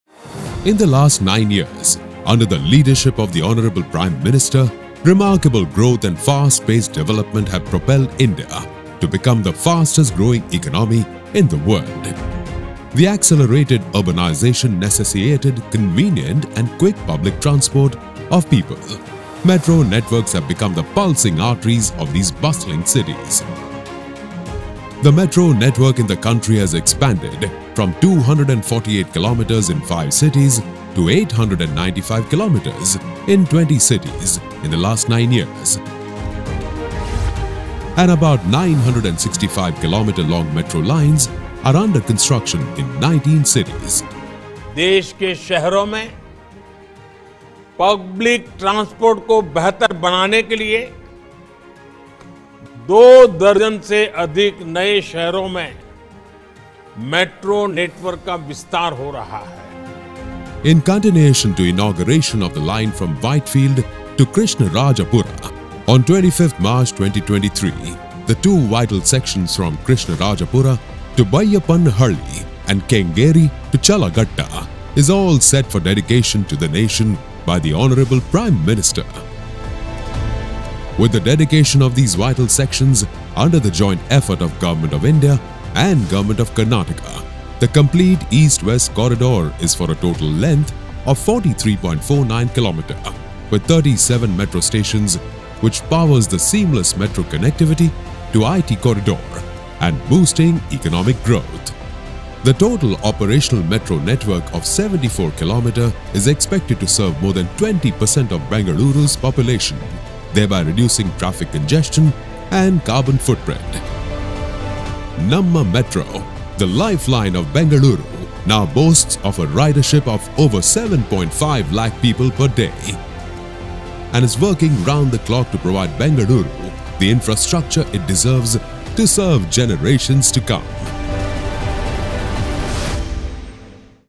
New Purple Line of Bengaluru Metro will facilitate Robust Urban Mobility & Connectivity
Prime Minister Narendra Modi will inaugurate and dedicate to the Nation the New Purple Line of the Bengaluru Metro which will facilitate connectivity from Krishnarajapura to Baiyappanahalli and Kengeri to Challaghatta.